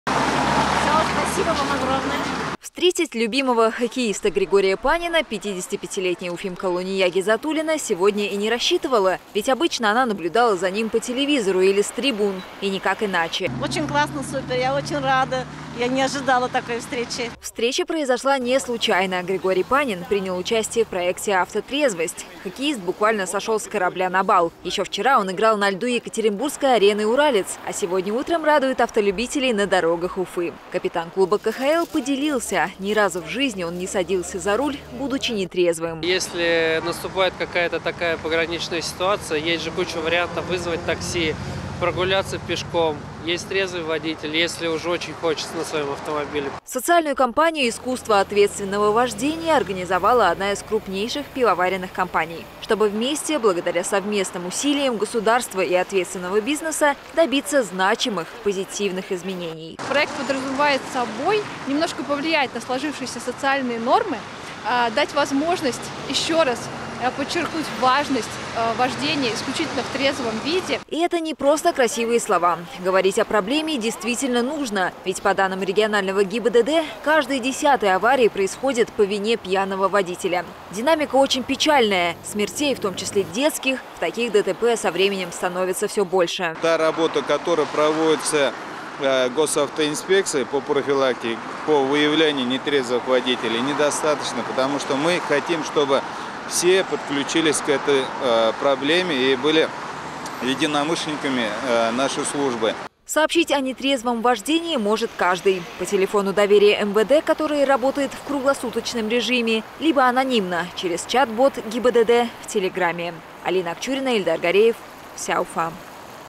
Каждое десятое ДТП в Башкирии происходит по вине пьяного водителя
Госавтоинспекция Башкирии совместно со спортсменами и бизнесменами города провела акцию «Автотрезвость»

О том, кто из известных личностей предупреждал автолюбителей о последствиях пагубной привычки, смотрите в нашем сюжете.

Новости Уфы в нашем телеграм-канале по ссылке